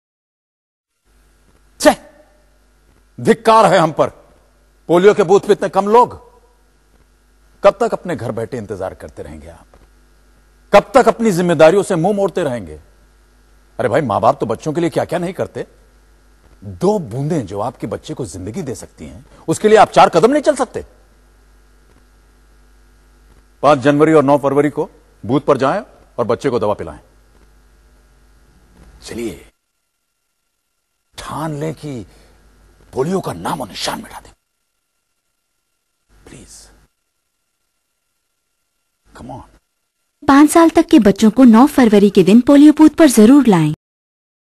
Polio Eradication Programme in India--AD
Polio Eradication Programme in India starring Amitabh Bachahan